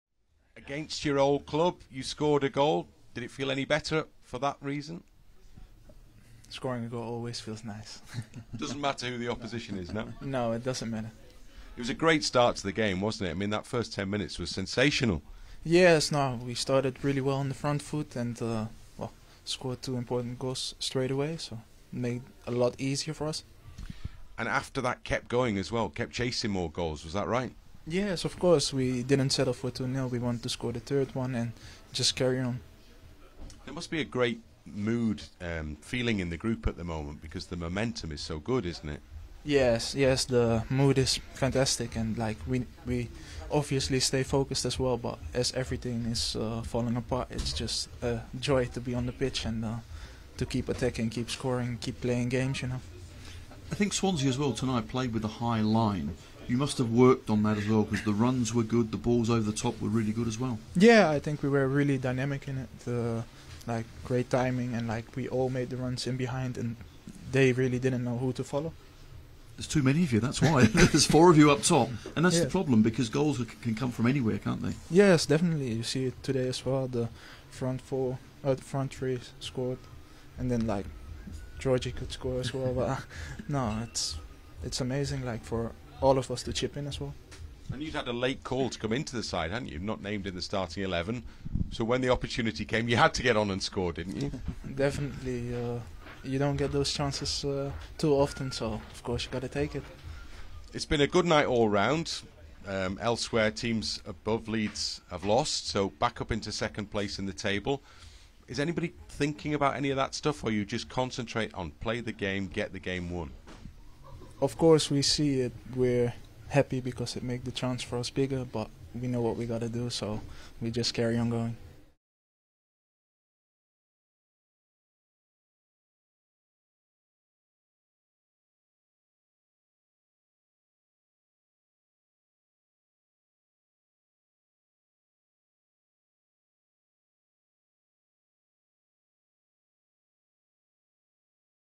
“Always nice to score” | Joel Piroe | Swansea City 0-4 Leeds United
Leeds United’s Joel Piroe gives his reaction to our 4-0 win over Swansea City at the Swansea.com Stadium in the EFL Championship.